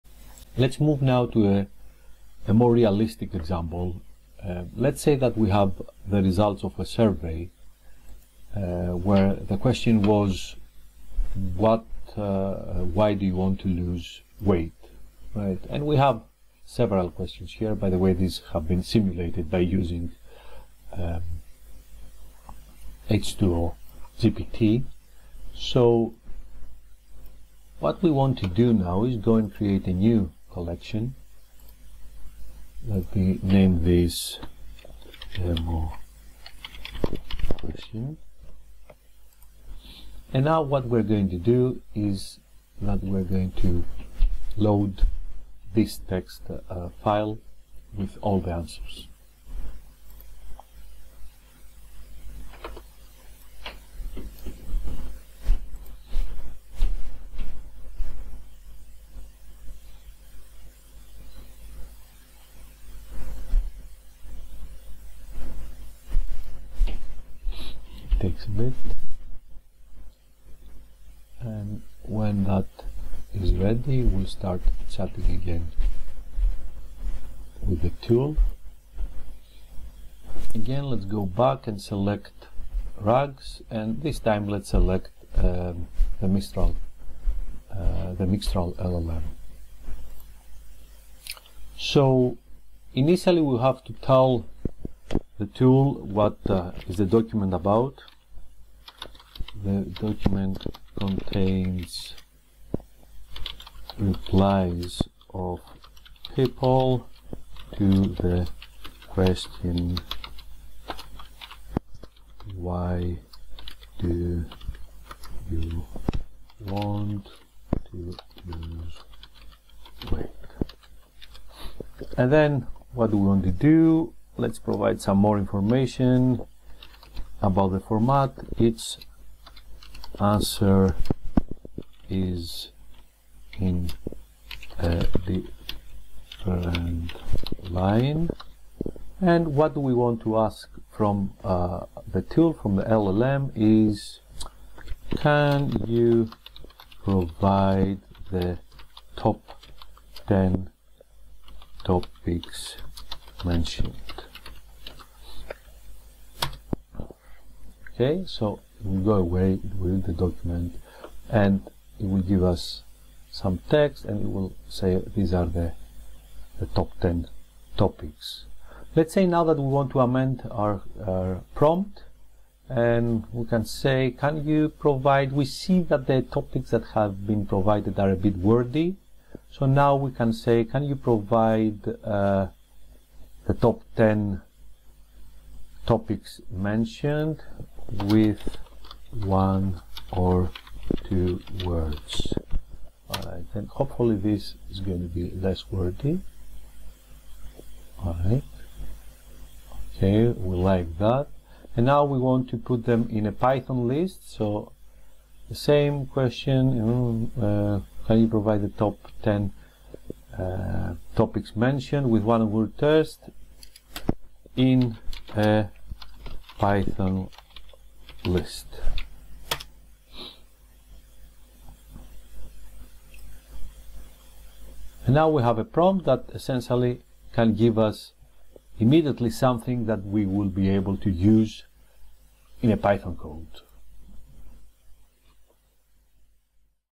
Real-World Survey Data Analysis with H2O GPTe RAG
We will explore a realistic use case of H2O GPTe RAG by analyzing survey responses.

 We will proceed by creating a new collection to load text files with survey answers, then use the RAG model to extract key topics.

In this video, you will see how the tool refines queries to provide concise and usable information, transforming survey data into actionable insights in a Python list.